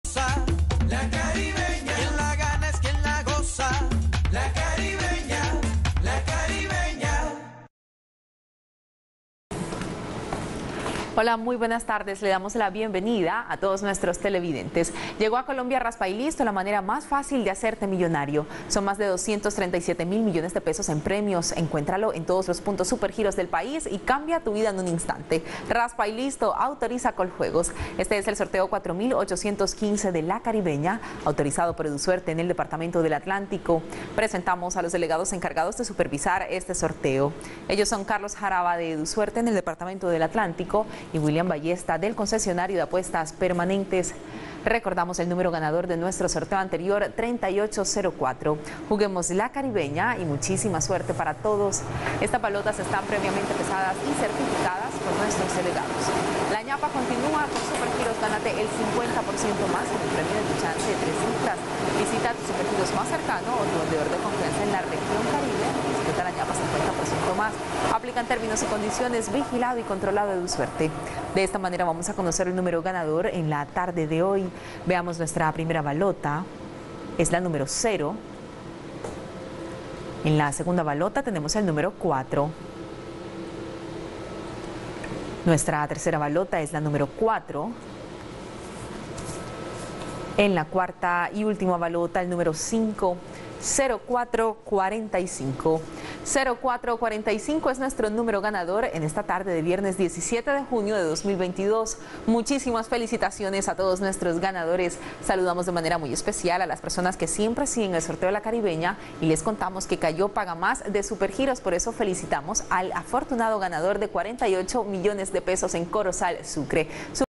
CARIBEÑA DÍA Resultado del VIERNES 17 DE JUNIO de 2022 ✅🥇🔥💰
Hola buenas tardes, te presentamos los resultados de las loterías y chances en Colombia, vamos con el Resultados del Chance Caribeña día del VIERNES 17 DE JUNIO de 2022.